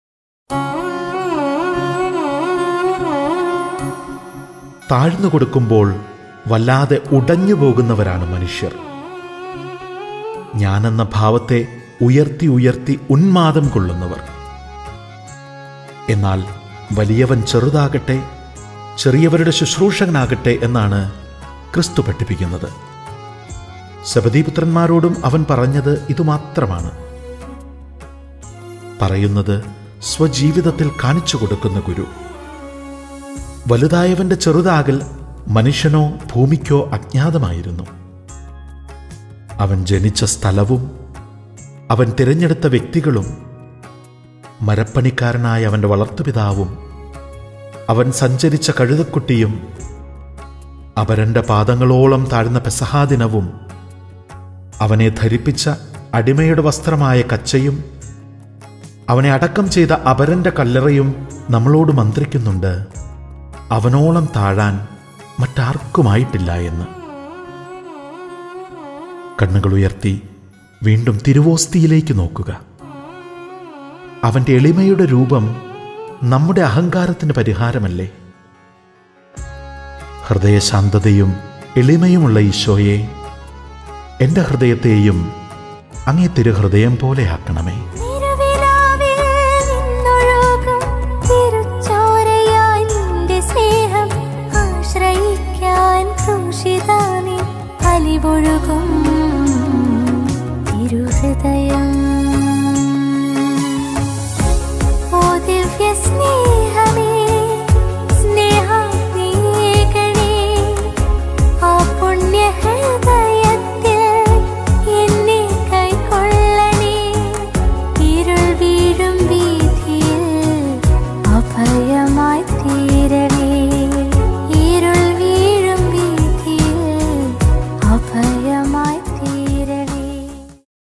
തിരുഹൃദയചിന്തകൾ 20/ Rosina Peety / Fr. Sanoj
*ജൂൺ 20*
തിരുഹൃദയചിന്തകൾ ഇരുപതാം ദിനം
*എളിമ നിറഞ്ഞ തിരുഹൃദയം*

Concept: Rosina Peety
Presentation: Fr. Sanoj Mundaplakkal

വലിയവൻ ചെറുതാകട്ടെ, ചെറിയവരുടെ ശുശ്രൂഷകർ ആകട്ടെ എന്നാണ് ക്രിസ്തു പഠിപ്പിക്കുന്നത്. അവൻ ജനിച്ച സ്ഥലവും അവൻ തിരഞ്ഞെടുത്ത വ്യക്തികളും മരപ്പണിക്കാരനായ അവന്റെ വളർത്തുപിതാവും അവൻ സഞ്ചരിച്ച കഴുതക്കുട്ടിയും അപരന്റെ പാദങ്ങളോളം താഴ്ന്ന പെസഹാദിനവും അവനെ ധരിപ്പിച്ച അടിമയുടെ വസ്ത്രമായ കച്ചയും അവനെ അടക്കം ചെയ്ത അപരന്റെ കല്ലറയും നമ്മളോട് മന്ത്രിക്കുന്നുണ്ട്, അവനോളം താഴാൻ മറ്റാർക്കും ആയിട്ടില്ലായെന്ന്. 🫀❤️